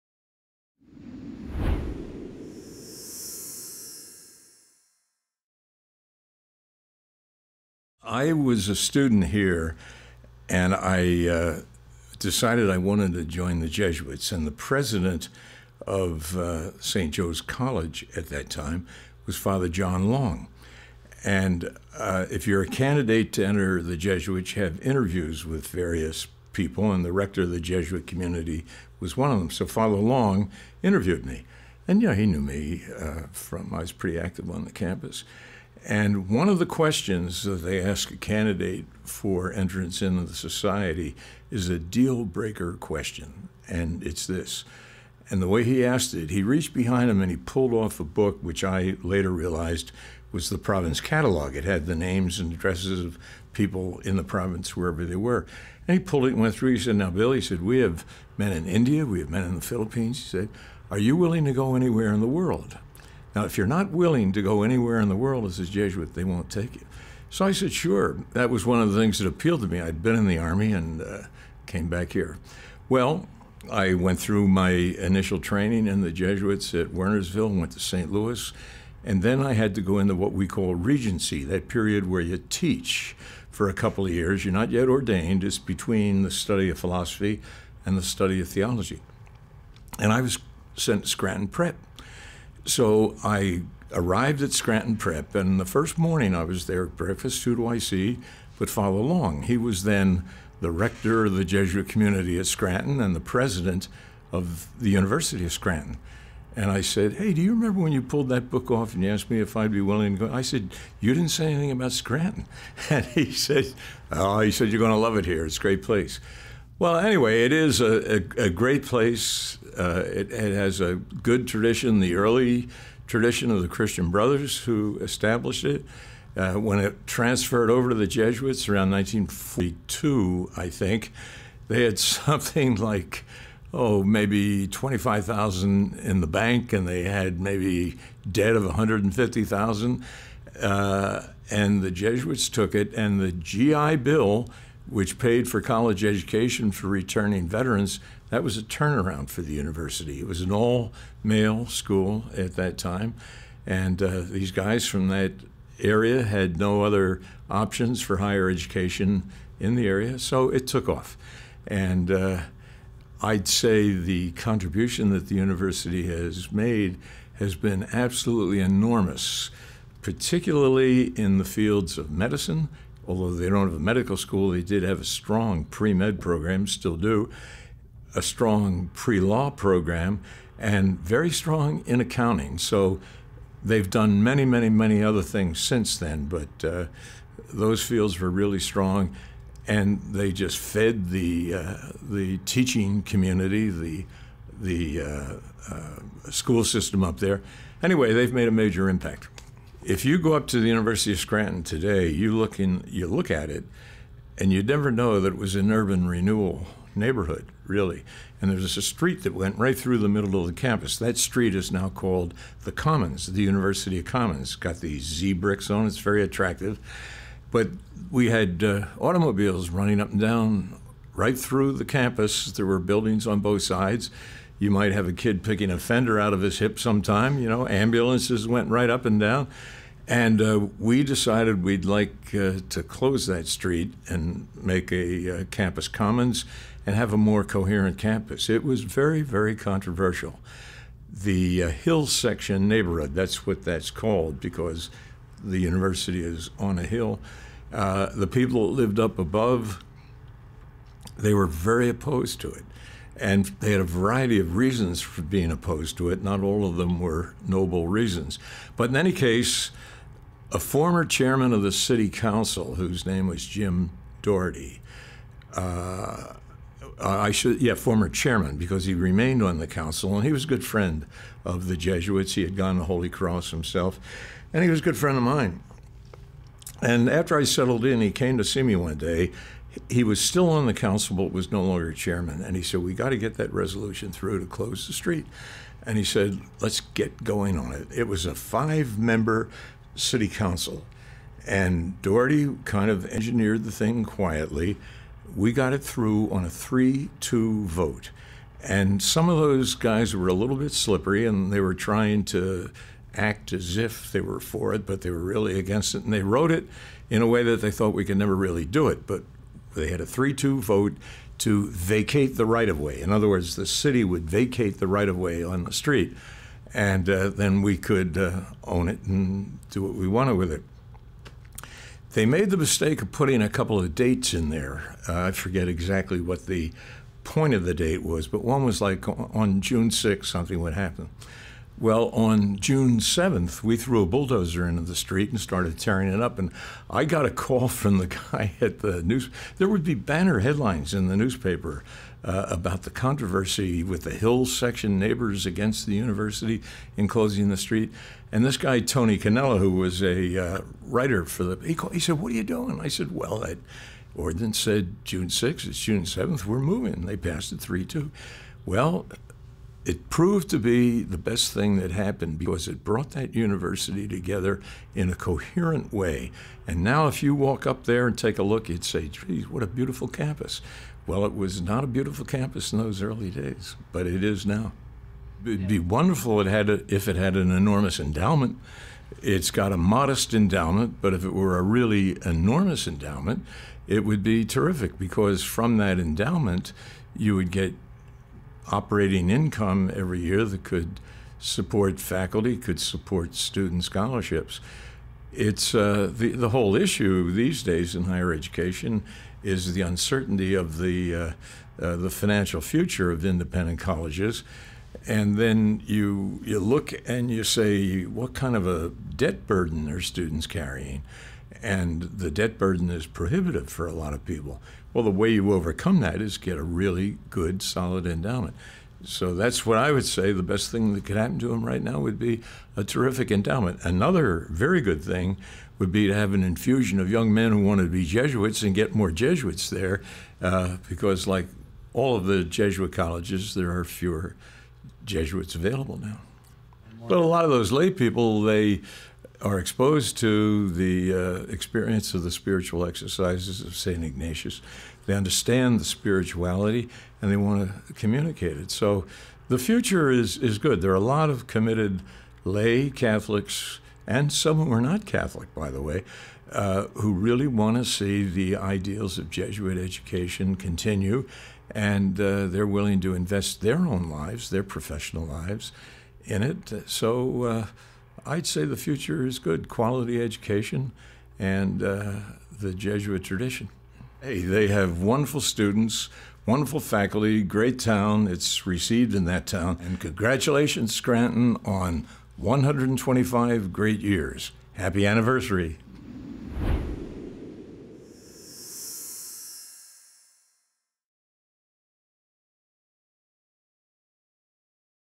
125th Anniversary Former University President's Message, William J. Byron, S.J., H '84
William J. Byron, S.J., H'84, served as University President from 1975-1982. After stepping down, he became the president of Catholic University of America and, later, was named president of St. Joseph's Preparatory School. He also held various other positions in academia, including at Loyola University New Orleans. Currently, he is University Professor of Business and Society at St. Joseph's University. He is also the author of over a dozen books on business ethics and leadership, among other topics.